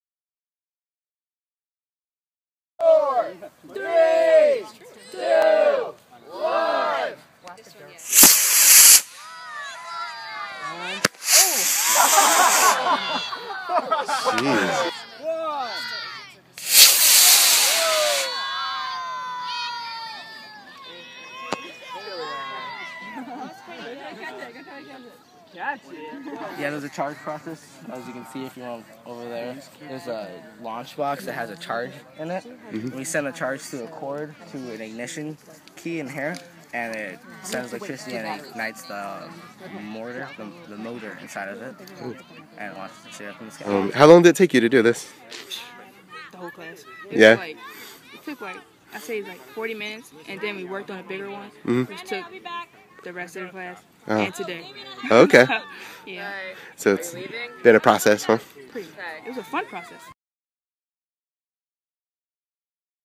Rocket launch at Cal Poly
More than 400 teens are attending a Cal Poly Engineering Possibilities in College camp this summer. They learned about the concepts of launching model rockets on Friday, July 11, 2014. | Video by Nick Wilson